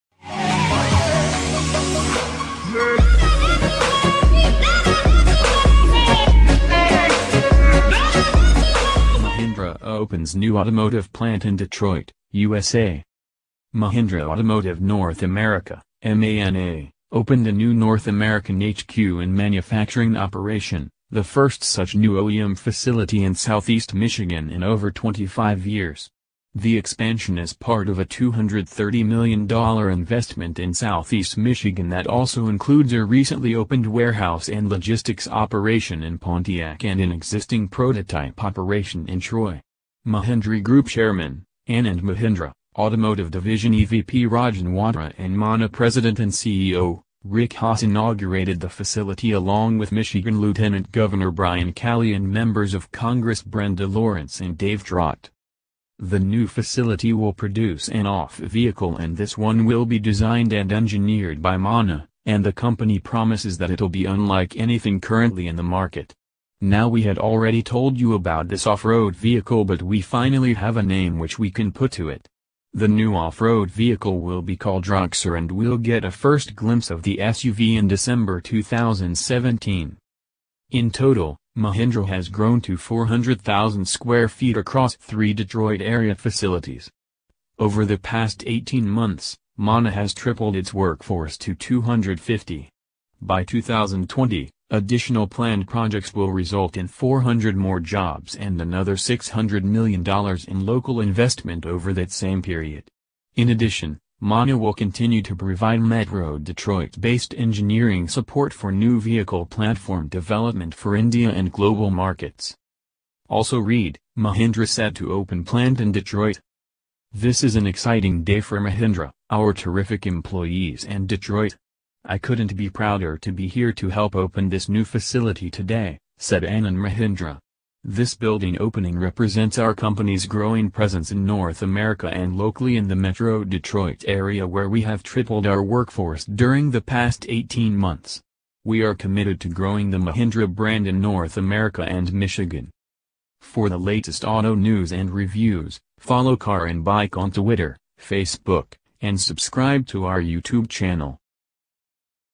Mahindra Opens New Automotive Plant In Detroit, USA
Mahindra Automotive North America (MANA) opened a new North American HQ and manufacturing operation, the first such new OEM facility in Southeast Michigan in over 25 years. The expansion is part of a $230 million investment in Southeast Michigan that also includes a recently-opened warehouse and logistics operation in Pontiac and an existing prototype operation in Troy. Mahindra Group Chairman, Anand Mahindra, Automotive Division EVP Rajan Wadhera and MANA President and CEO, Rick Haas inaugurated the facility along with Michigan Lieutenant Governor Brian Calley and Members of Congress Brenda Lawrence and Dave Trott.

The new facility will produce an off- vehicle and this one will be designed and engineered by MANA, and the company promises that it'll be unlike anything currently in the market. Now we had already told you about this off-road vehicle but we finally have a name which we can put to it. The new off-road vehicle will be called Roxor and we'll get a first glimpse of the SUV in December 2017.

In total, Mahindra has grown to 400,000 sq. ft. across three Detroit area facilities. Over the past 18 months, MANA has tripled its workforce to 250. By 2020, additional planned projects will result in 400 more jobs and another $600 million in local investment over that same period. In addition, MANA will continue to provide Metro Detroit-based engineering support for new vehicle platform development for India and global markets.